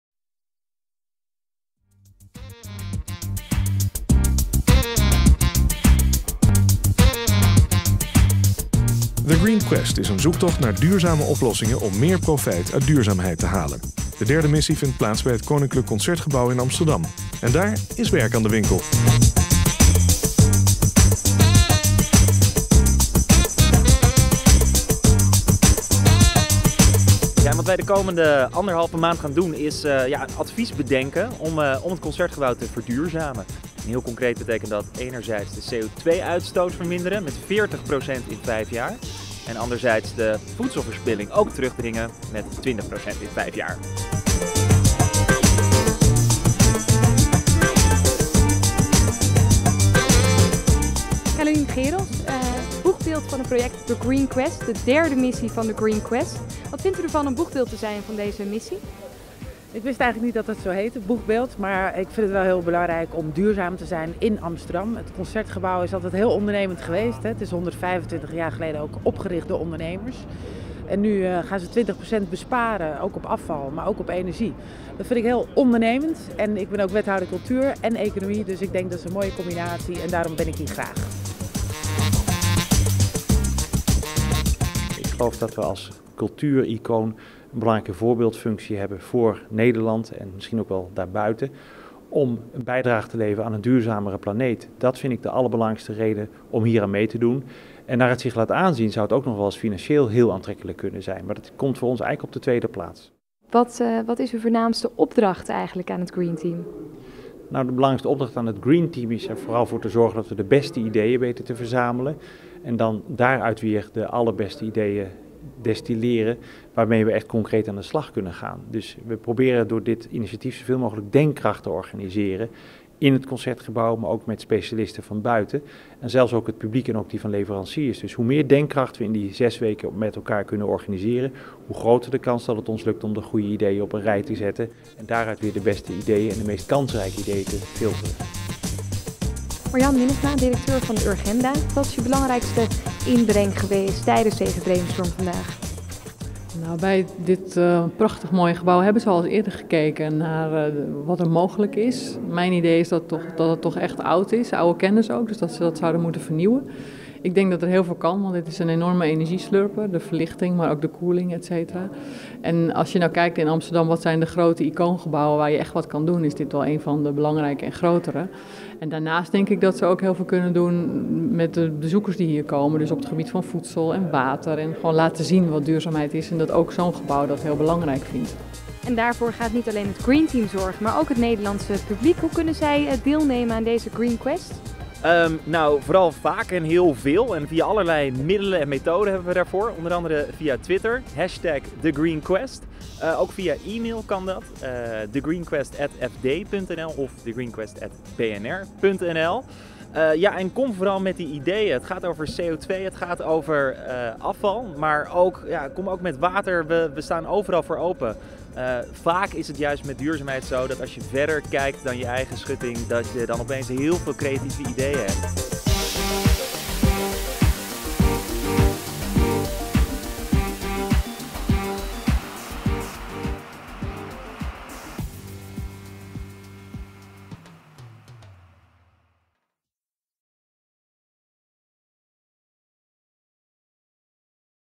The Green Quest verduurzaamt Het Koninklijk Concertgebouw Amsterdam
Amsterdam, 16 september 2013 - De derde missie van The Green Quest is officieel van start. Tijdens een brainstorm in de bekende bubble, maakte het Green Team kennis met de duurzaamheidsdoelstellingen van Het Koninklijk Concertgebouw.

Onder belangstelling van het voorbij komende publiek maakten duurzaamheidsexperts Marjan Minnesma (Urgenda), Ralien Bekkers (VN-jongerenvertegenwoordiger), Marieke van der Werf (energie deskundige) en Maurits Groen (duurzaam ondernemer) kennis met de directie van het Concertgebouw. Na een uitgebreide rondleiding spraken de experts toegestroomde pers toe en gaven zij officieel het startschot voor de derde missie.

Eindrapportage

Op 28 oktober zal het Green Team haar bevindingen aan de directie van het Concertgebouw presenteren. De duurzaamheidsexperts nemen de komende weken de tijd om alle ideeën nader te onderzoeken. Zij doen daarbij een beroep op het publiek.

Ideeën

Heb je een suggestie voor The Green Quest? Twitter dan thegreenquest of stuur een email naar . De 10 beste inzendingen nodigen we uit bij de eindpresentatie te zijn op 28 oktober in Het Koninklijk Concertgebouw.